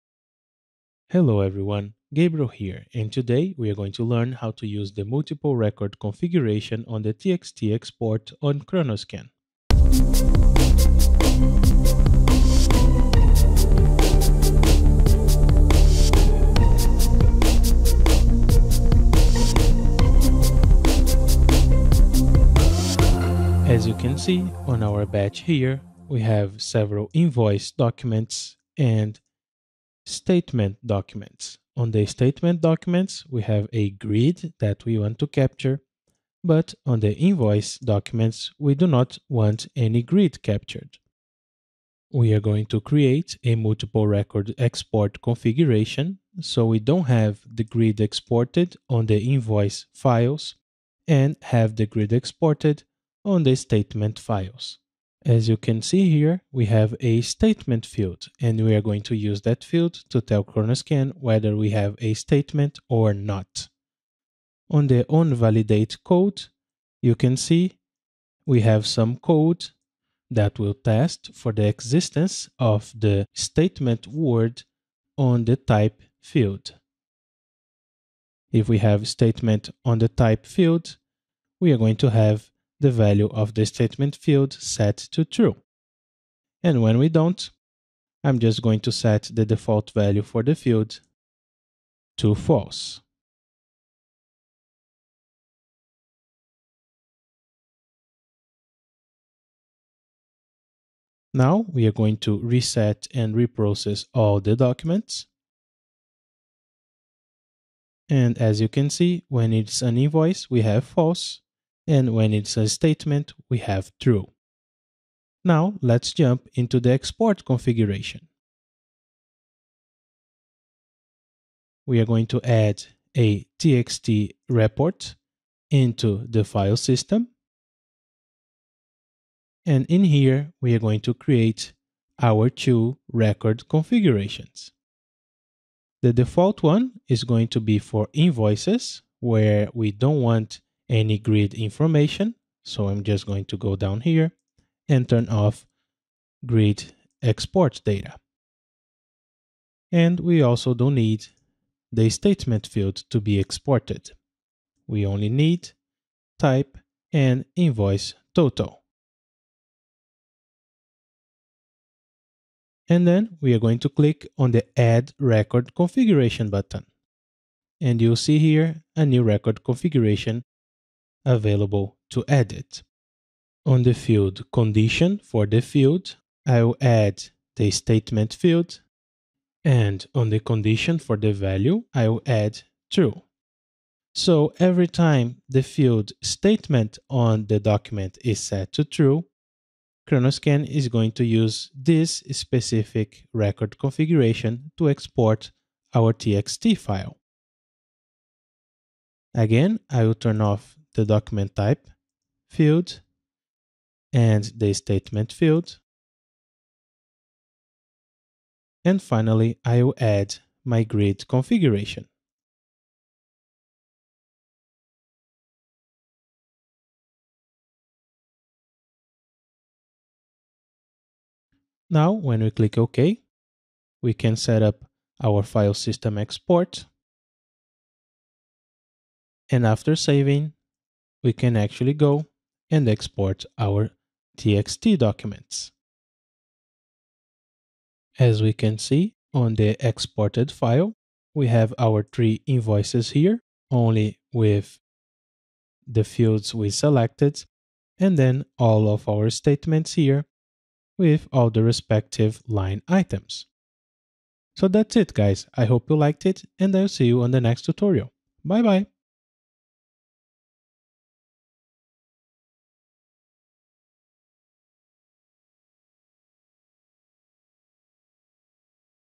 Multiple Record Export Configuration on ChronoScan
On this video we will learn how to use the new multiple record export configuration on the TXT export module to export grid fields only for the document of a certain type.